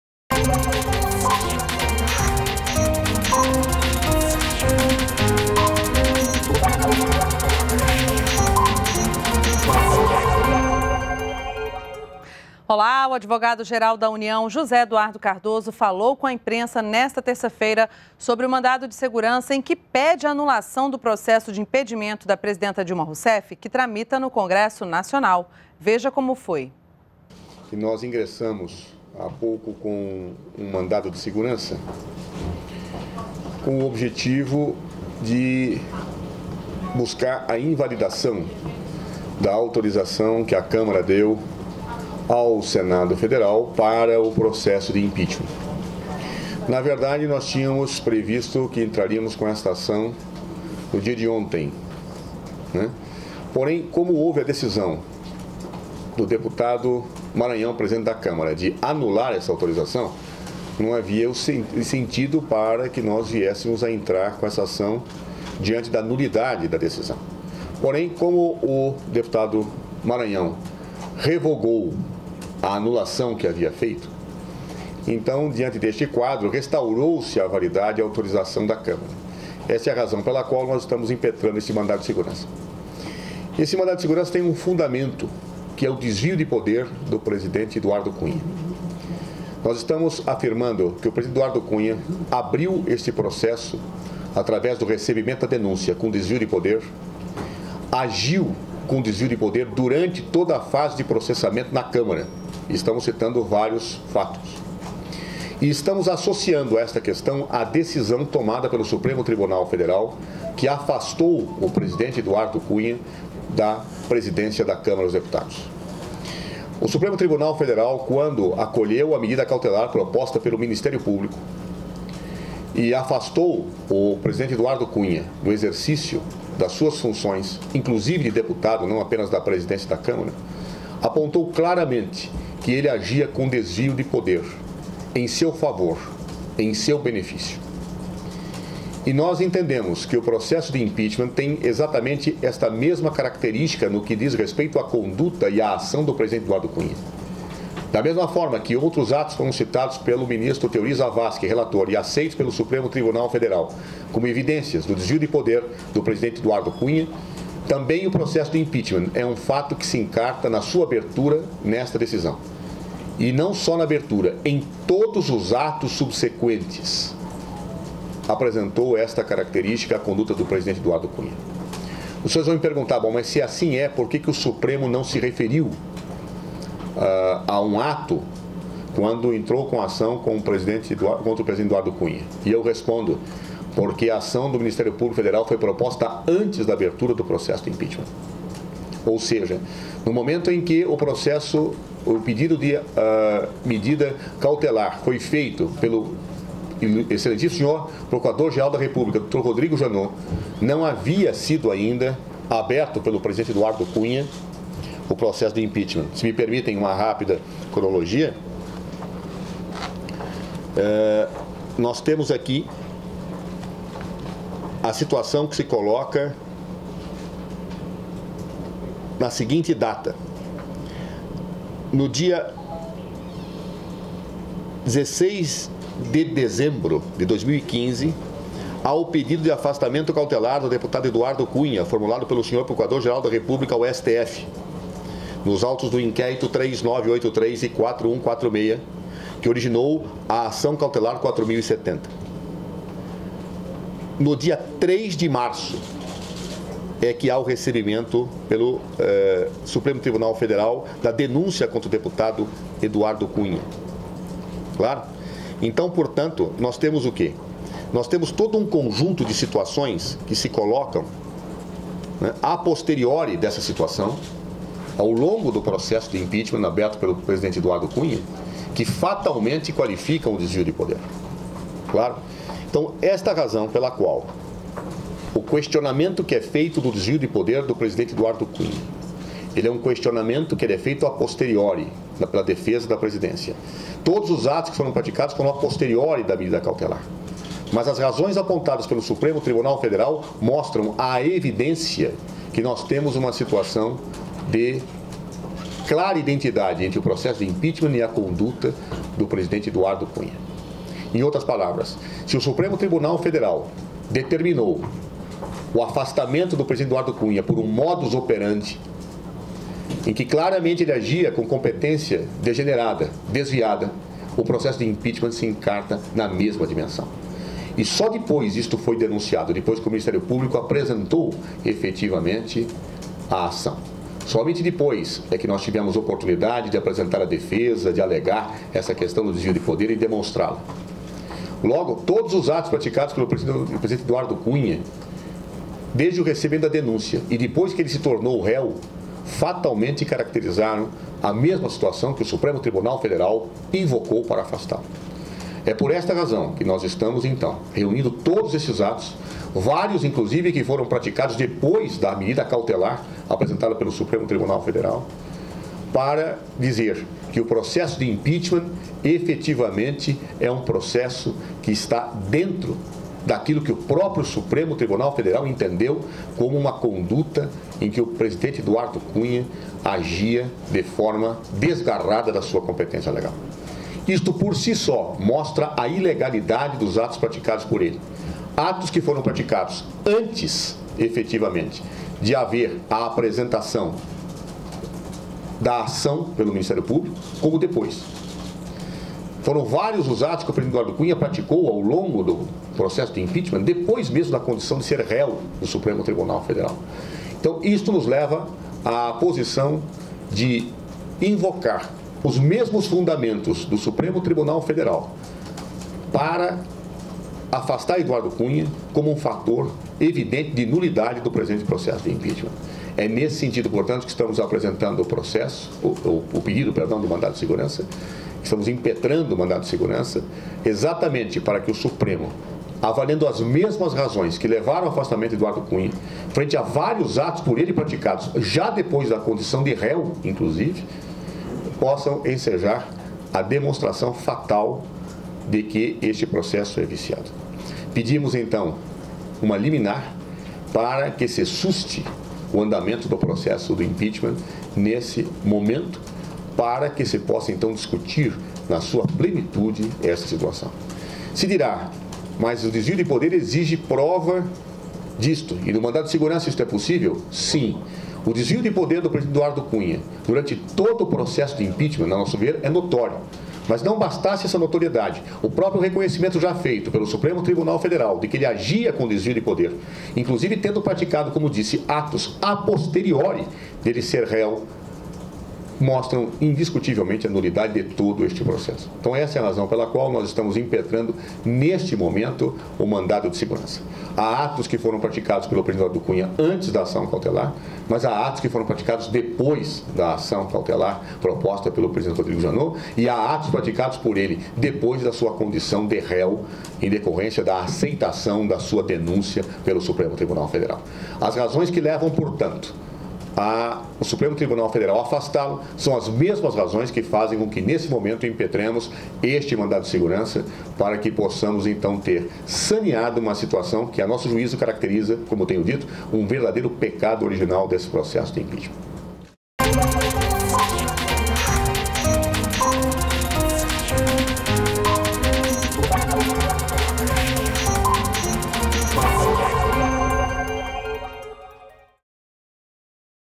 Advogado-geral da União fala sobre mandato de segurança que pede anulação do processo de impedimento
REPÓRTER NBR - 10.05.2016: O advogado-geral da União, José Eduardo Cardozo, afirmou nesta terça-feira (10/05) que afastar do cargo um presidente da República sem que ele tenha praticado crime de responsabilidade seria uma ruptura institucional que demonstraria a fragilidade da democracia brasileira. A declaração foi dada durante entrevista coletiva na qual o ministro deu detalhes sobre mandado de segurança impetrado no Supremo Tribunal Federal (STF) para pedir, em caráter liminar, a suspensão do processo de impeachment.